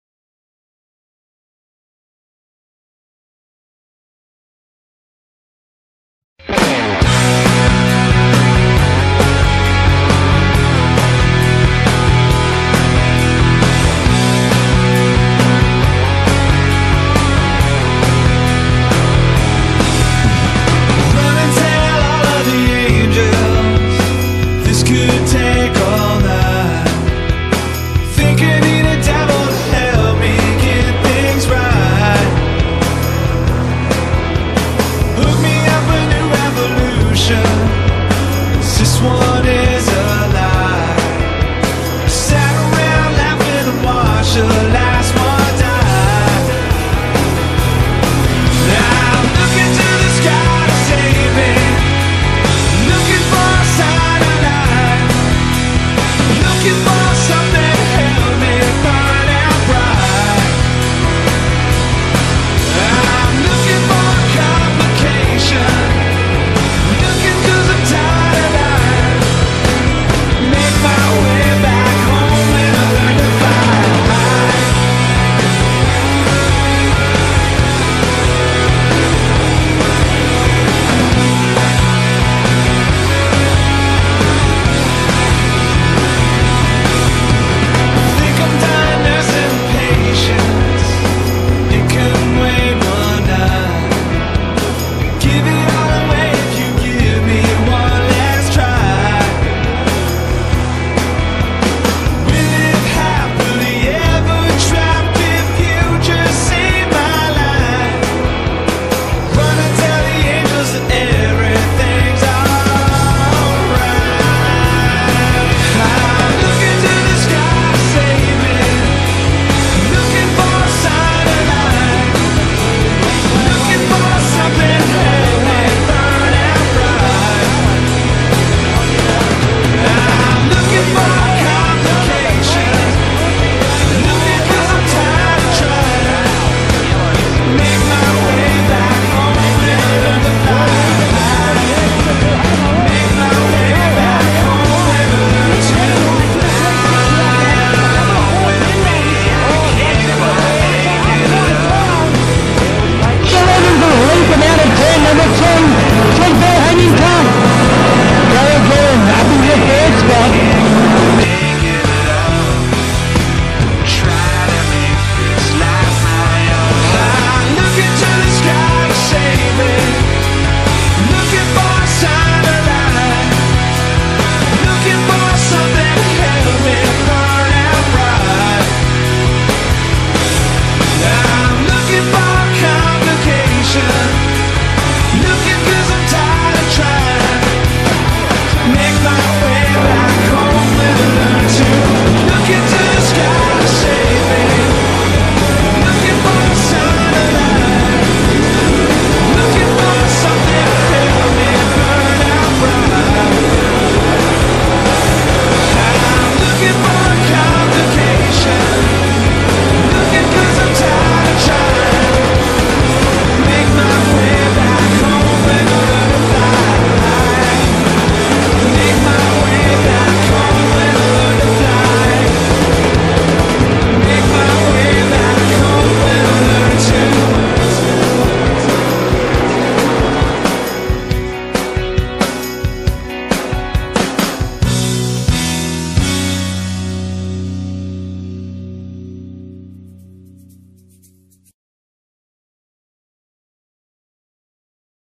SA 360 SPRINTCAR SEASON 2013/2014
SEE THE MIGHT 360s FIGHT IT OUT AT MURRAY BRIDGE SPEEDWAY AND SPEEDWAY CITY THROUGH OUT THE 2013/2014 SPEEDWAY SEASON. SEE THE HIGHS AND LOWS OF SPEEDWAY RACING.